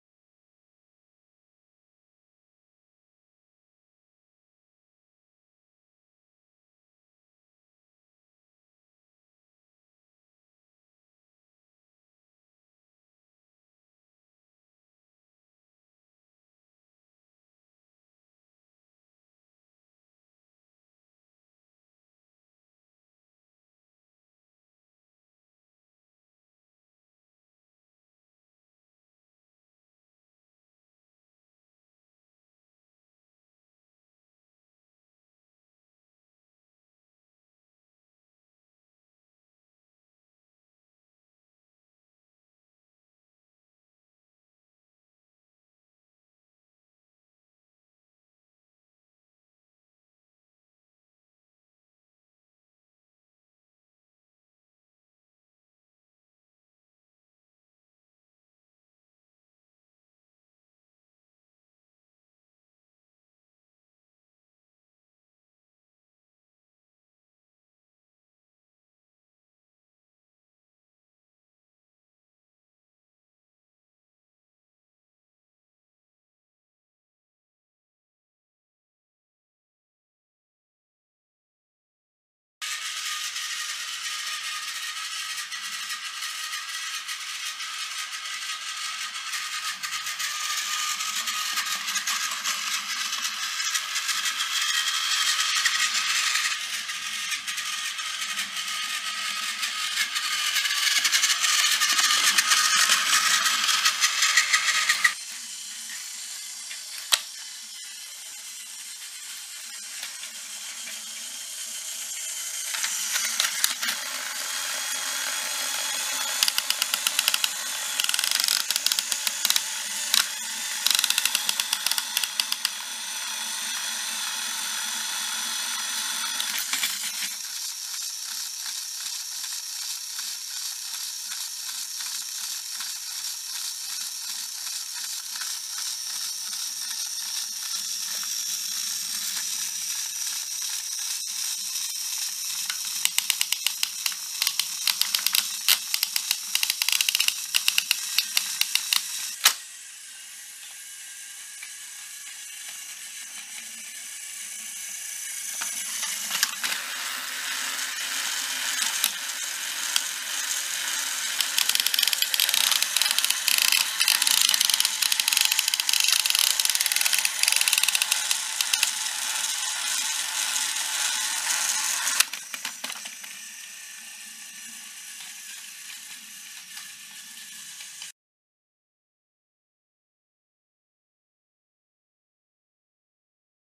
【発掘！？珍プラレール】 大きな転車台＆専用レール
「きかんしゃトーマスドキドキマウンテンと大きな転車台セット」というトイザらス限定で発売されていたレールです。
通常品と異なり、炭水車付きのテンダー式蒸気機関車にも対応しているのでとても重宝されています。